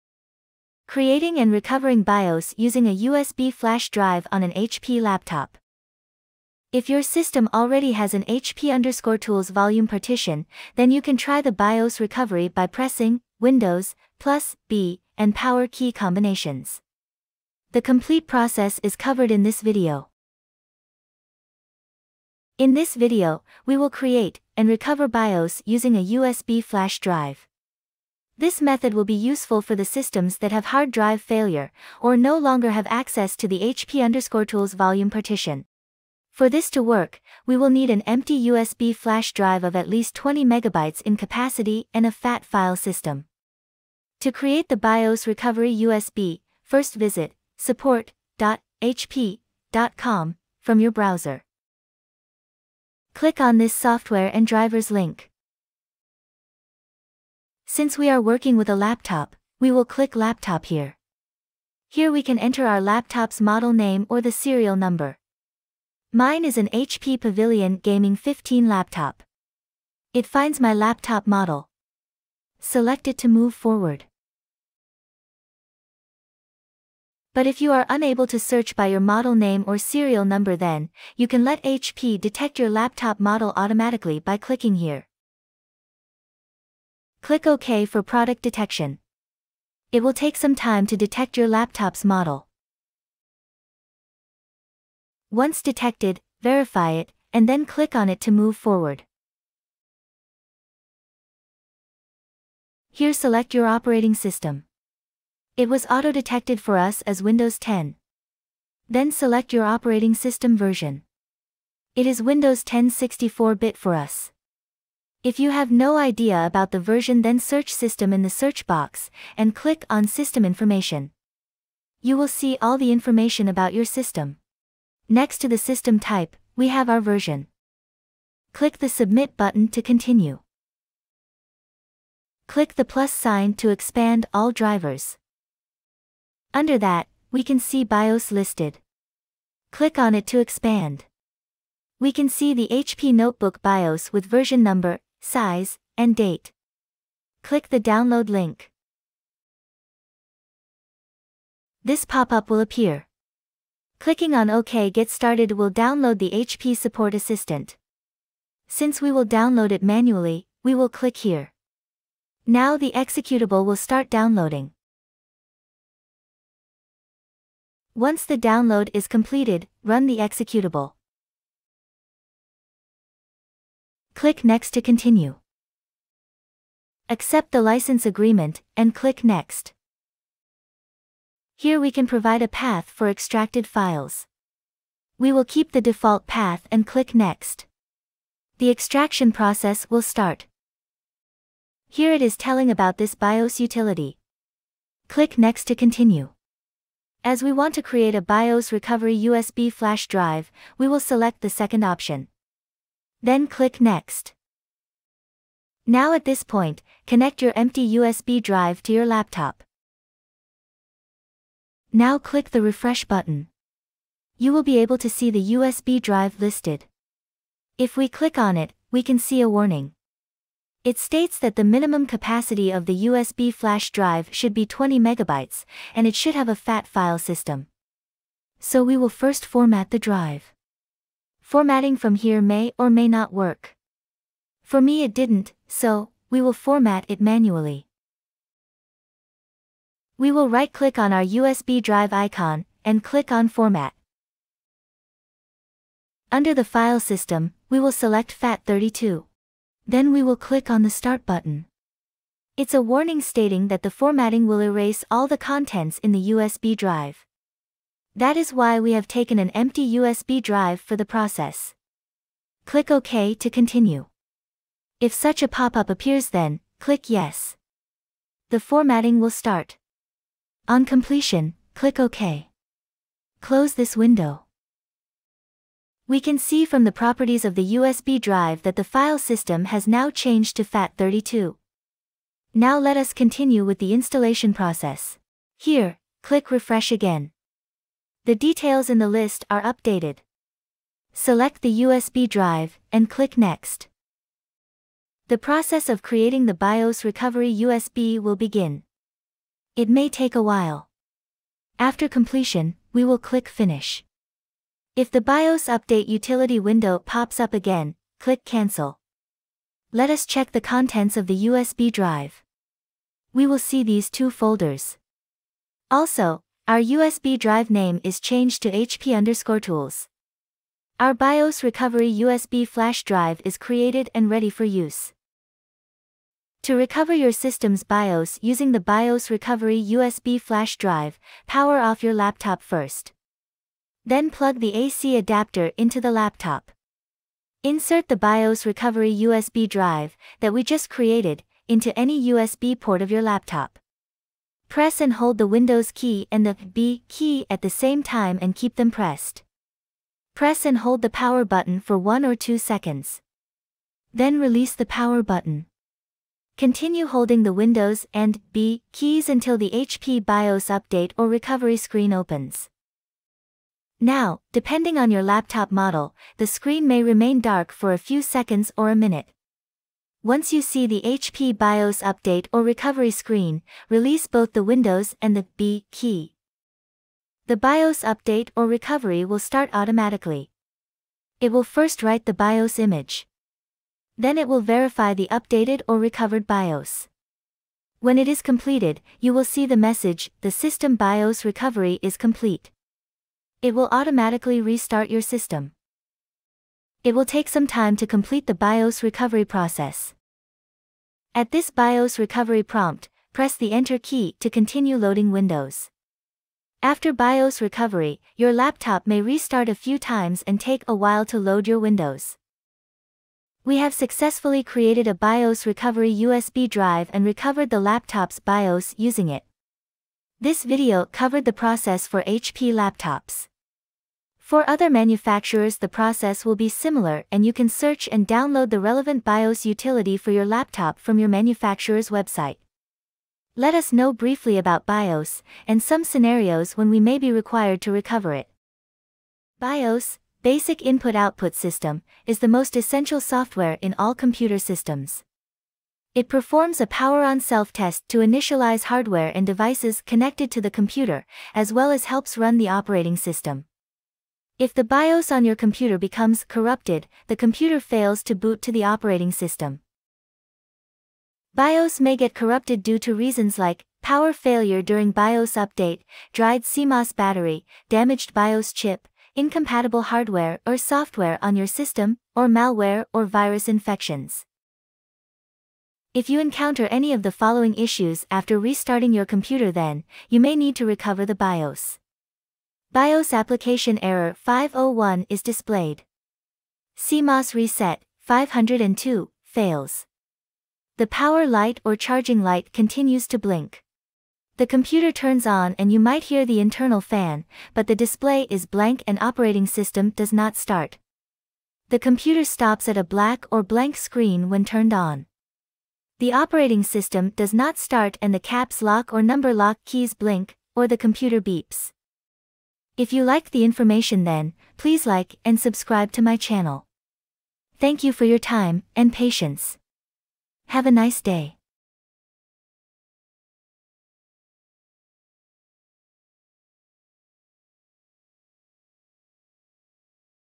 Creating BIOS Recovery USB Flash Drive | Recovering BIOS using USB | HP Laptop
BIOS (Basic input output system) is the most essential software in all computer systems. It performs a power-on-self-test to initialize hardware and devices connected to the computer, as well as helps run the operating system. If the BIOS on your computer becomes corrupted, the computer fails to boot to the Operating system.

If your system already has an HP_TOOLS volume partition, then you can try the BIOS recovery by pressing, Windows, "B" and power key combinations. In this video, we will create, and recover BIOS using a USB flash drive. This method will be useful for the systems that have hard drive failure or no longer have access to the HP_TOOLS volume partition. For this to work, we will need an empty USB flash drive of at least 20 megabytes in capacity and a FAT32 file system.

The process is simple and easy to follow. I hope the video helps.